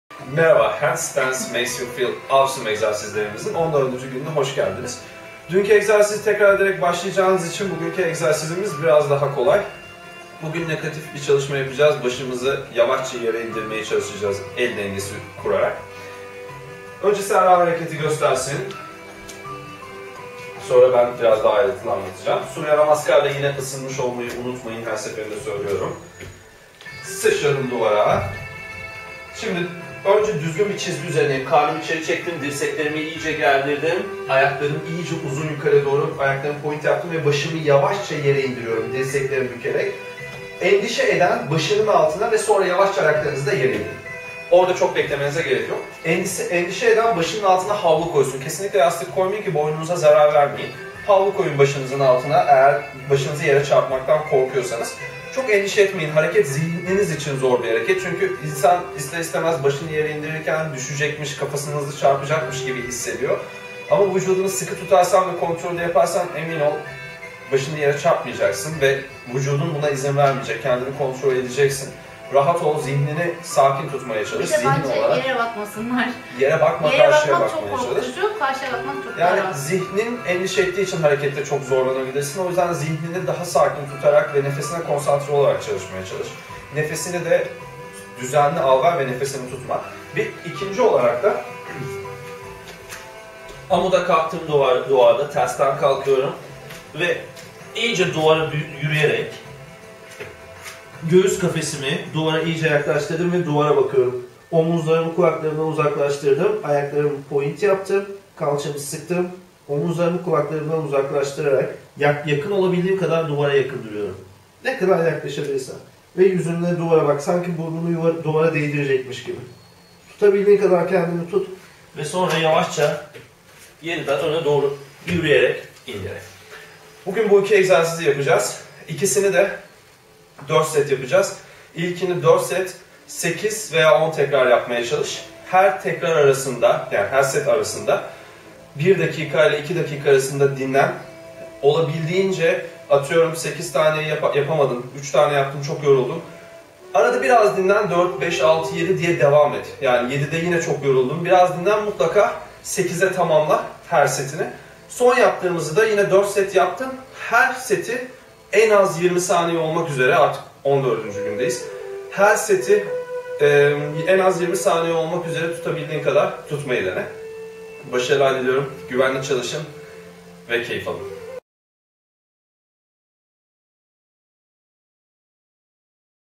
Gün 14 handstandmakesyoufeelawesome (El dengesi) egzersizleri
Çetin Çetintaş ile handstandmakesyoufeelawesome 14. günü ile el dengesi çalışmasına tam gaz devam! 30 gün boyunca aralıksız hergunun egzersizleri ile çalışmanıza devam ederek muhteşem sonuçlara ulaşmaya hazır olun. Egzersizlere başlarken Suryanamaskar ile ısınıp ardından esneme hareketlerini yapıp, el bileklerini ısıtmayı unutma! 1 gün önceki egzersizleride başta tekrar edin ve bugüne öyle başlayın. Güvenli ve keyifli çalışmalar!